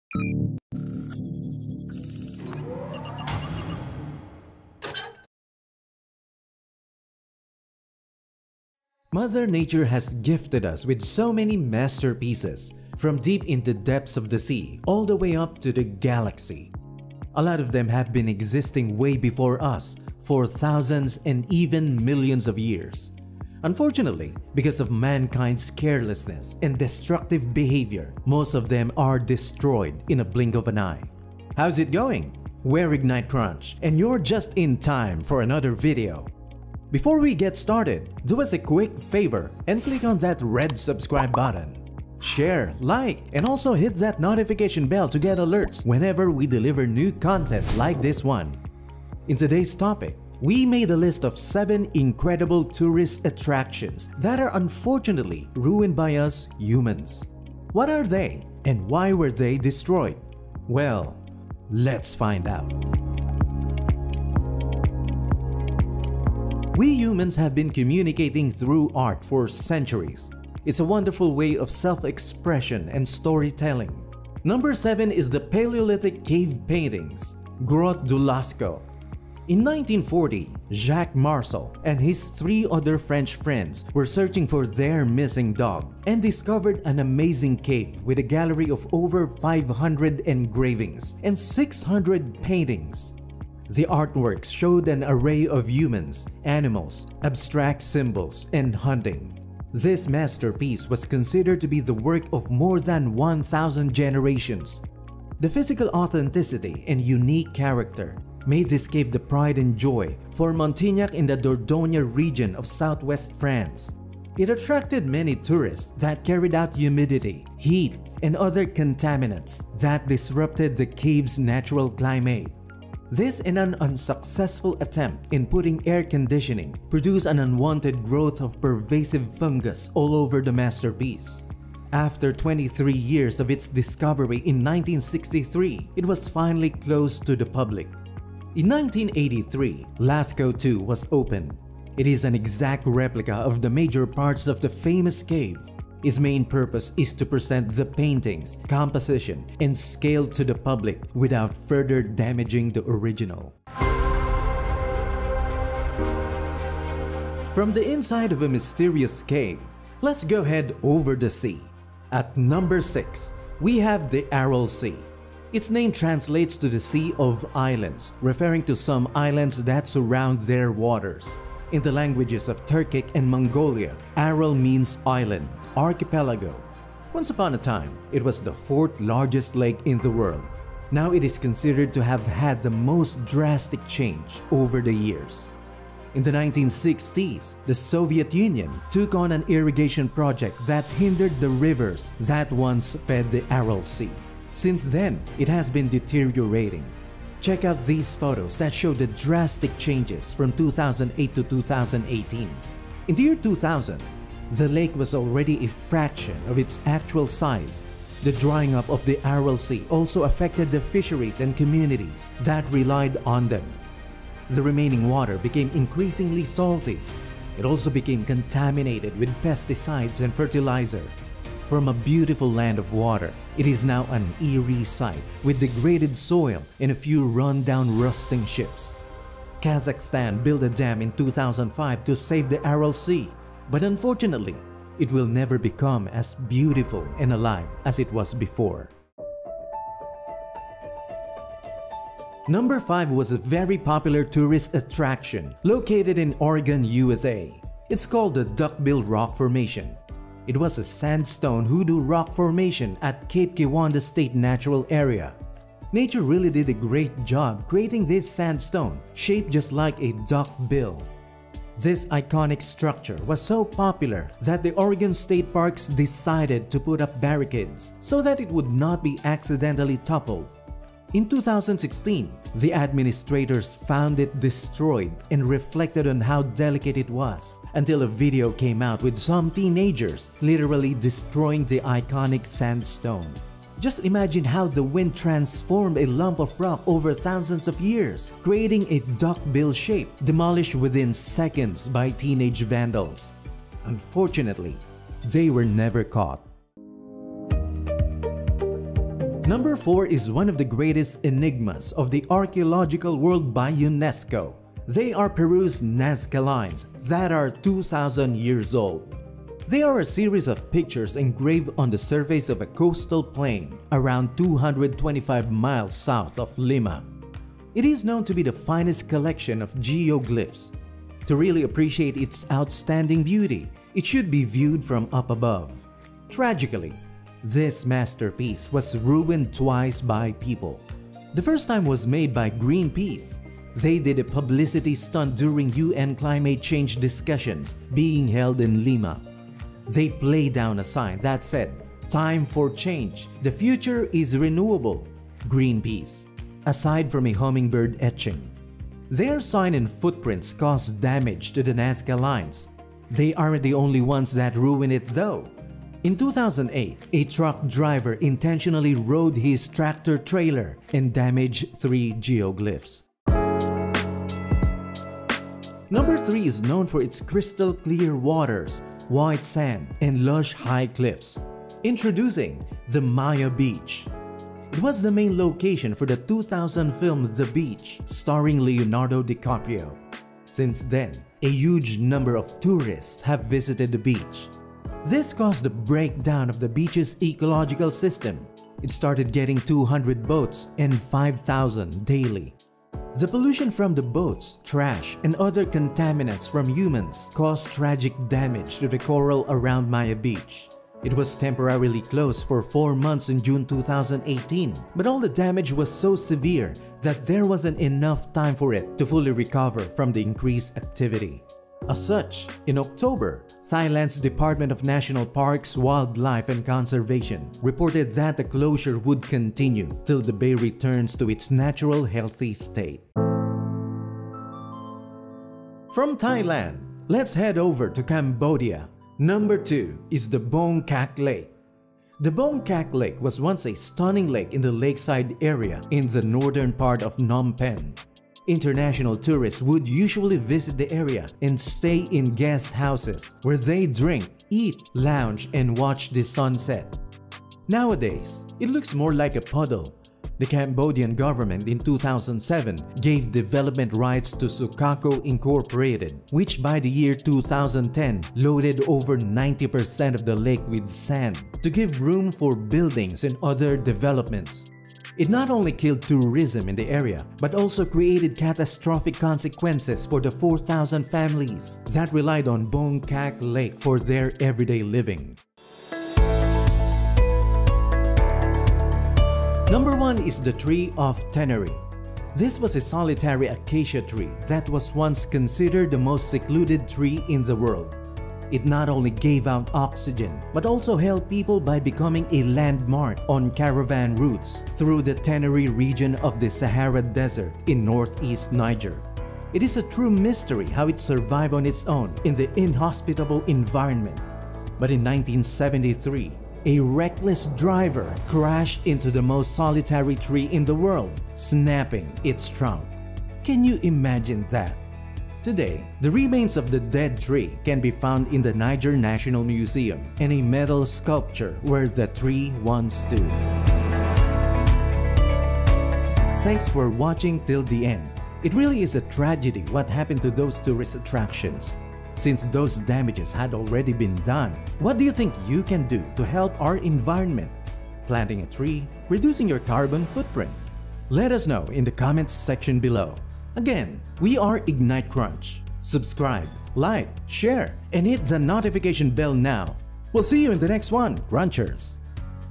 7 Incredible Tourist Attractions Ruined by People
Mother Nature has gifted us with so many natural masterpieces, from deep in the depths of the sea all the way up to the sky. Unfortunately, we humans take them for granted and that causes their destruction. Today, we are going to show 7 incredible tourist attractions ruined by people.

Timestamps:
0:00 7 Incredible Tourist Attractions Ruined by People
1:18 Tourist Attraction No 7:  Paleolithic Cave Paintings
2:44 Tourist Attraction No 6:  Aral Sea
4:04 Tourist Attraction No 5:  Duckbill Rock Formation
5:04 Tourist Attraction No 4:  Nazca Lines
6:14 Tourist Attraction No 3:  Maya Beach
7:10 Tourist Attraction No 2:  Boeung Kak Lake
8:07 Tourist Attraction No 1: Tree of Ténéré tree of tenere